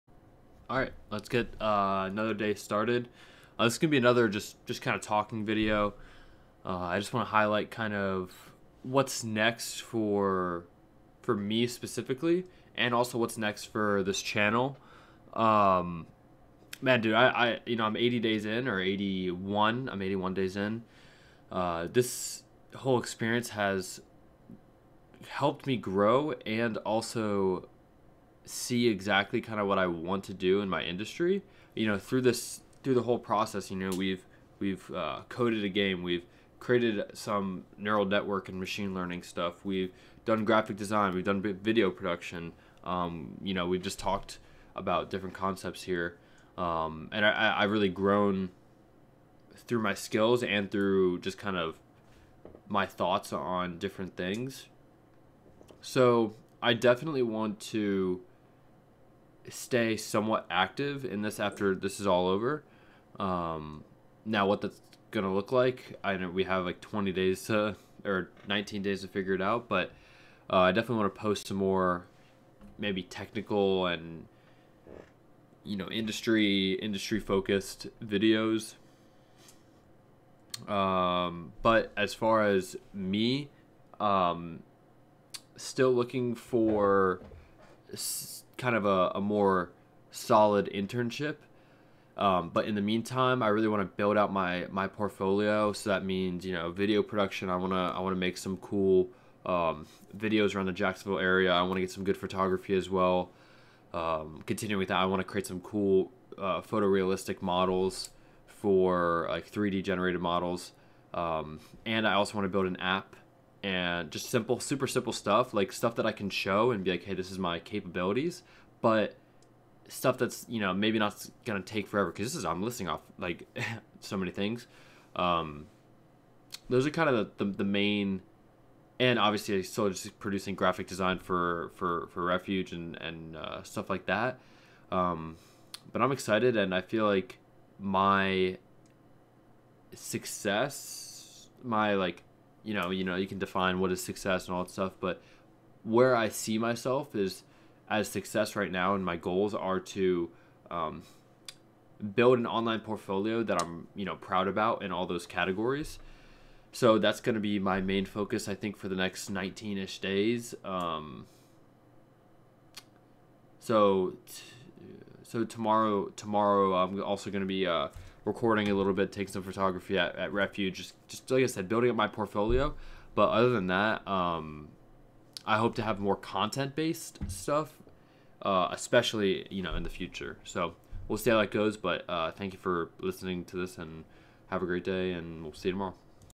The Future of This Channel
Day 81 of a 100-day challenge to release an uncut/raw video detailing the current creative and technical processes occurring in my life.

Topics Highlighted: a conversation about my goals and aspirations in the near future for myself and this channel.